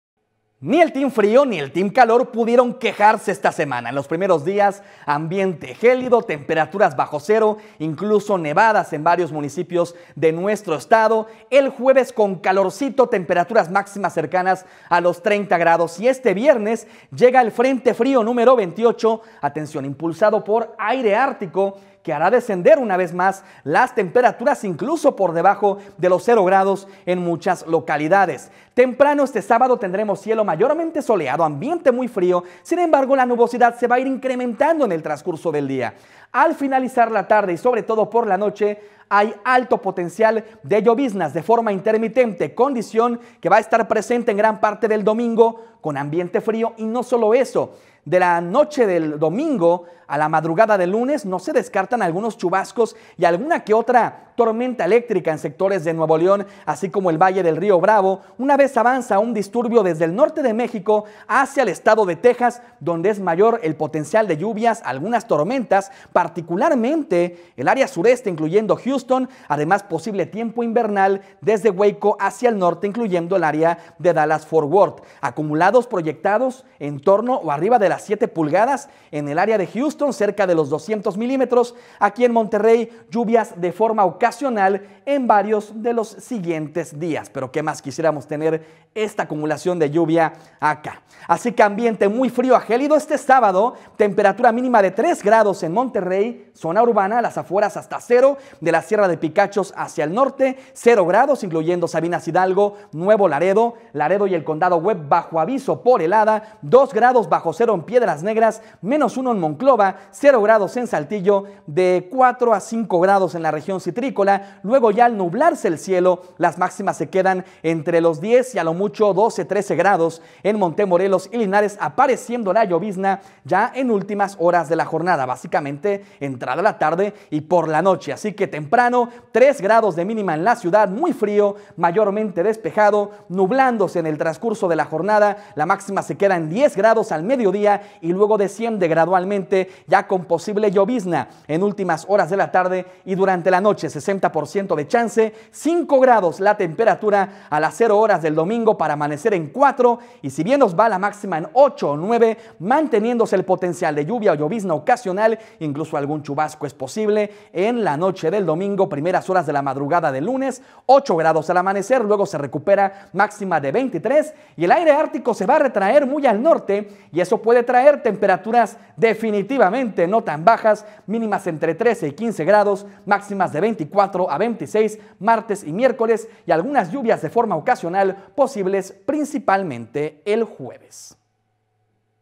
Nuevo frente frío y lluvias este fin de semana en Monterrey
Frente frío 28 llega a Nuevo León con otro descenso de temperatura, a la espera de mínimas cercanas a 0° temprano el sábado en la región, y posibles lloviznas en Monterrey a partir de la tarde, extendiéndose hasta el lunes. David Faz con el pronóstico para el fin de semana.